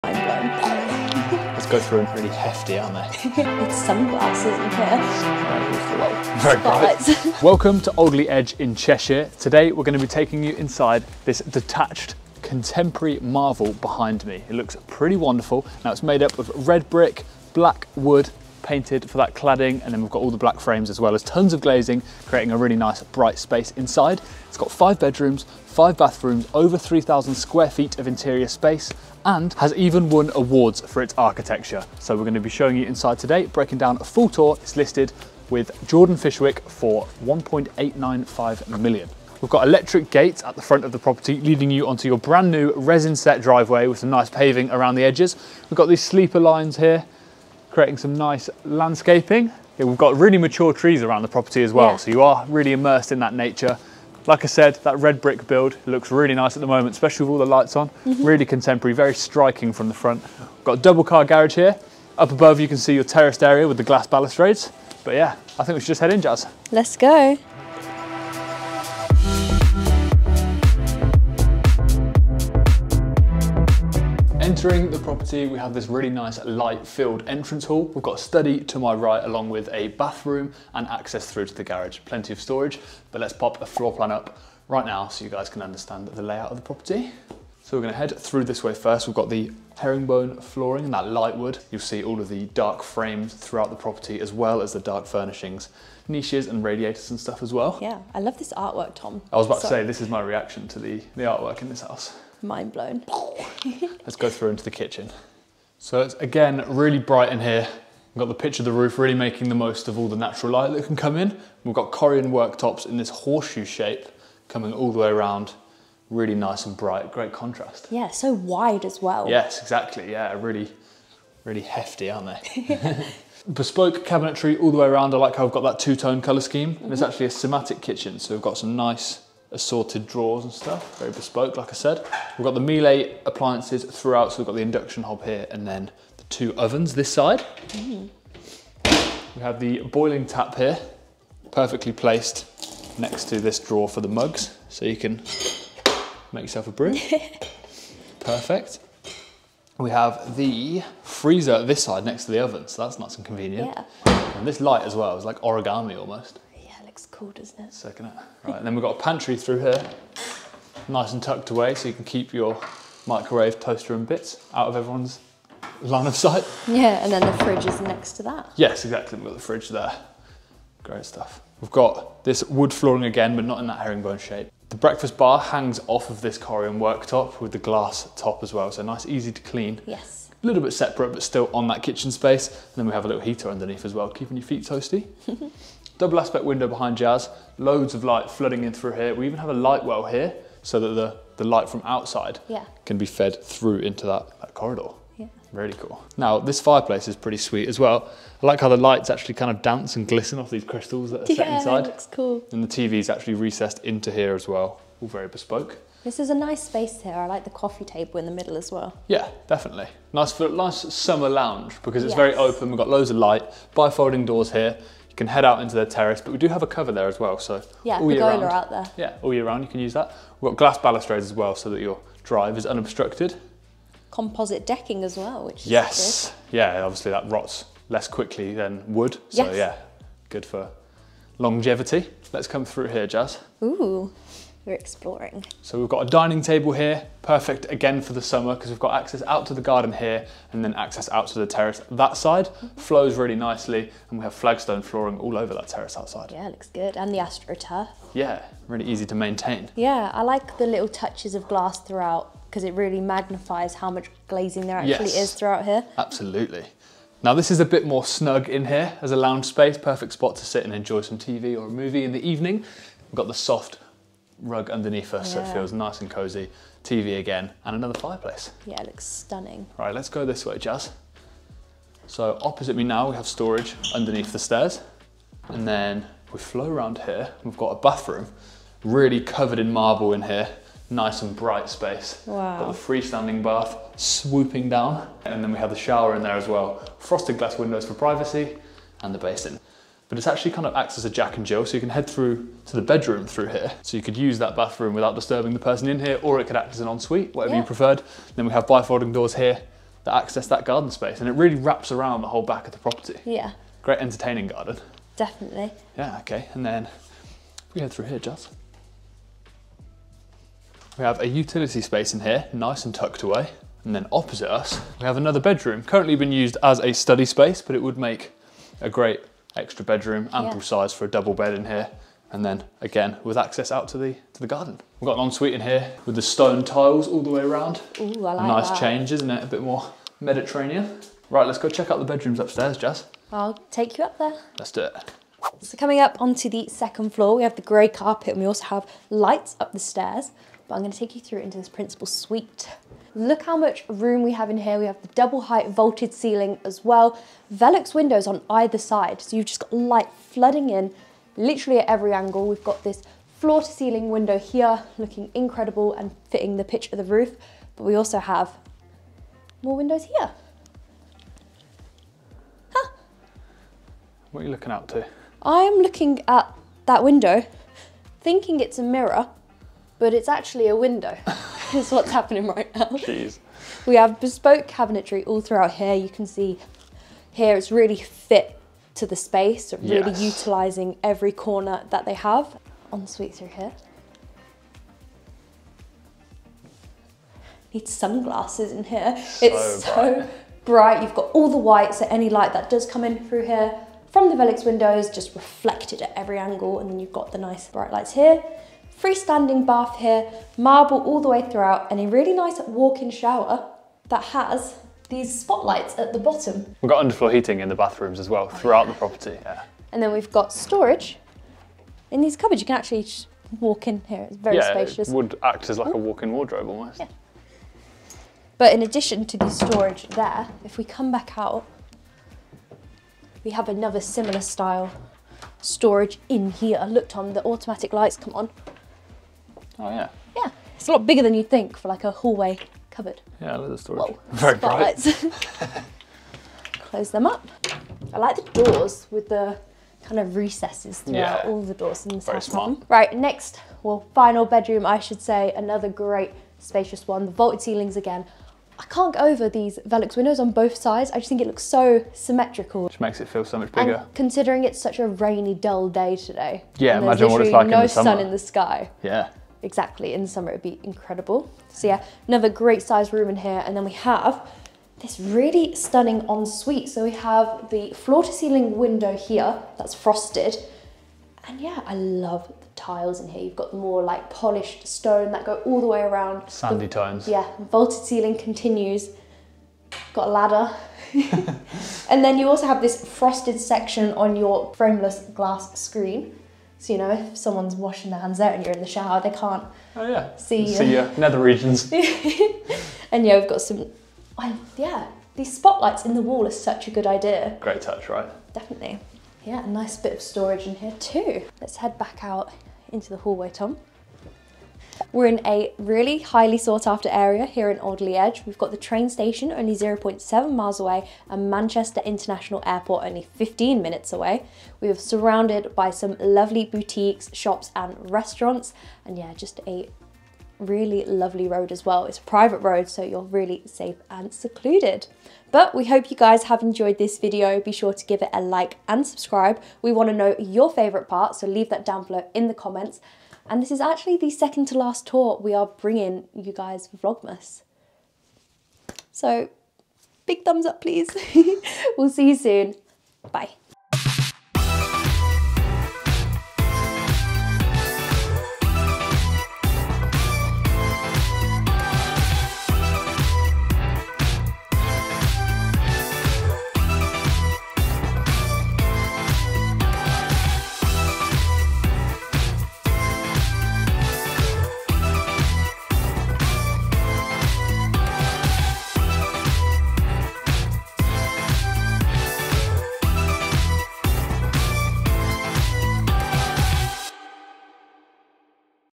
£1,895,000 in Alderley Edge | Cheshires Finest Village
Do you have a luxury or unique home for us to feature?

Listed with: Jordan Fishwick | Victoria Horseman

Would you like a free no obligation mortgage consultation?

Episode Chapters:
00:00 Intro
01:07 Entrance
01:44 Kitchen
03:10 Living Room
03:57 Dining
04:17 Lounge
04:37 Bathroom
04:53 Bedroom 1
05:13 Bedroom 2
05:40 Principal Suite
07:19 Bedroom 4